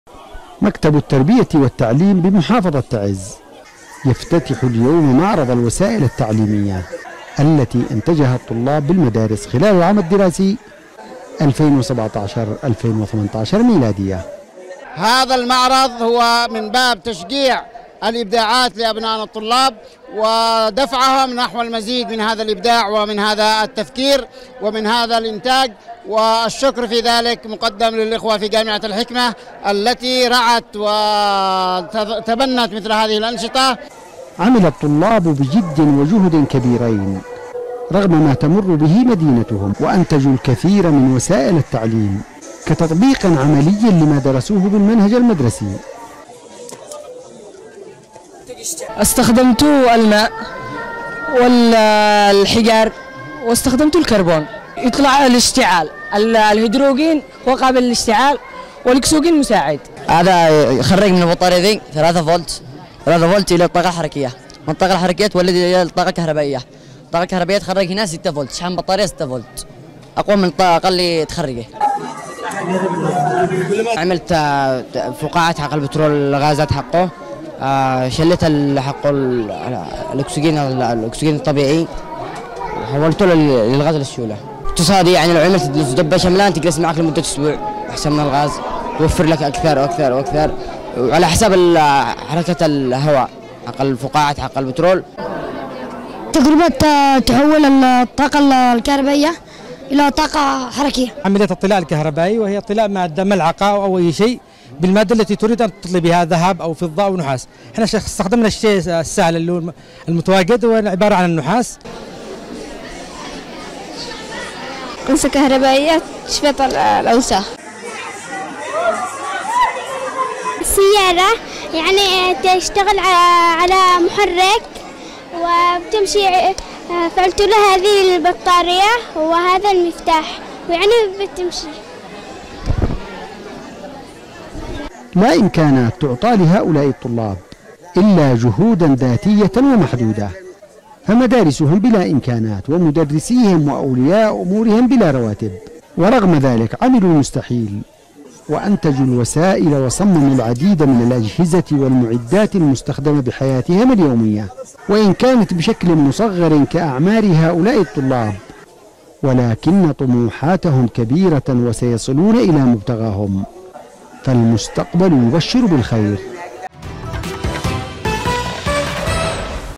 تعز.. افتتاح معرض الأنشطة الفنية والوسائل التعليمية
بحضور وكيل محافظة تعز ومدير مكتب التربية والتعليم في المحافظة افتتح اليوم في مدينة تعز معرض الأنشطة النفية الوسائل التعليمية، وقد احتوى المعرض على جملة من ابداعات الطلاب في العديد من المجالات ..